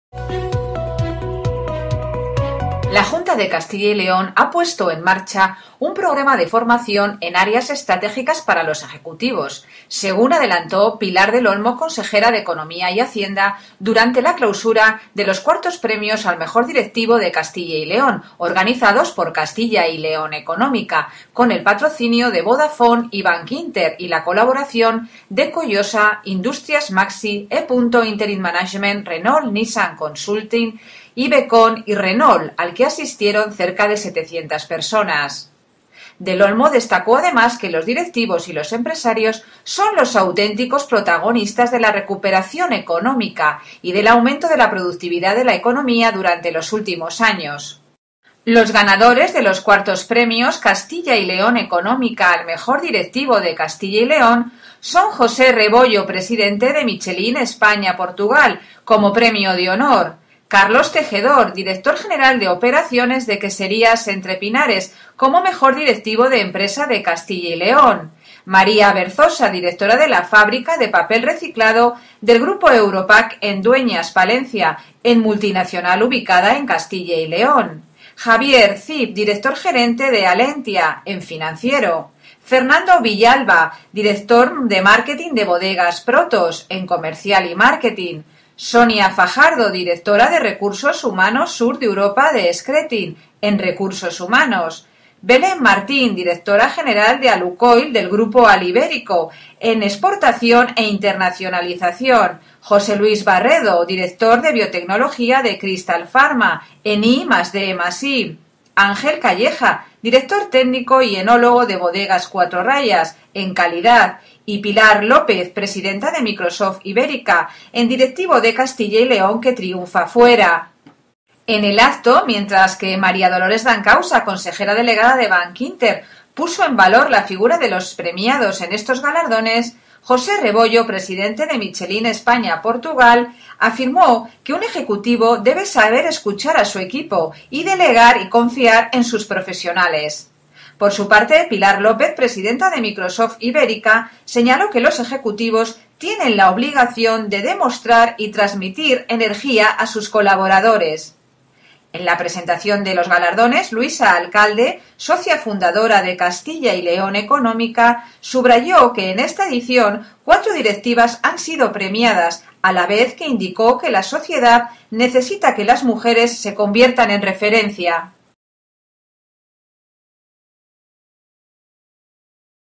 IV Premios Castilla y León Económica al Mejor Directivo
La Junta de Castilla y León ha puesto en marcha un programa de formación en áreas estratégicas para los ejecutivos, según adelantó Pilar del Olmo, consejera de Economía y Hacienda, durante la clausura de los IV Premios al Mejor Directivo, organizados por la revista Castilla y León Económica, con el copatrocinio de Vodafone y Bankinter y la colaboración de Collosa, Industrias Maxi, Epunto Interim Management, Renault-Nissan Consulting, Ibecon y Renault y al que asistieron 700 personas.